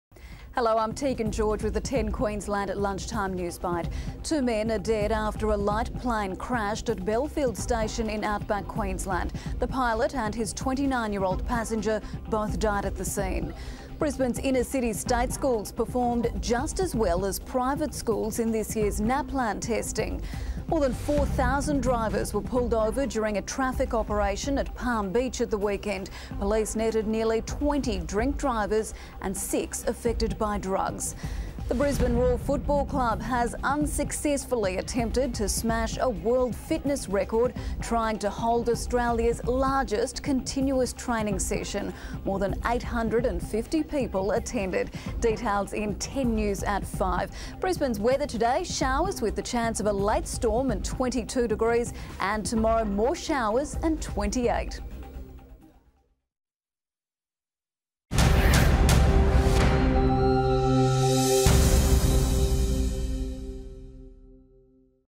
Queensland Lunchtime Newsbyte
(16/09/2013): Two men have died after a light plane crash in outback Queensland. That and more in our lunchtime newsbyte.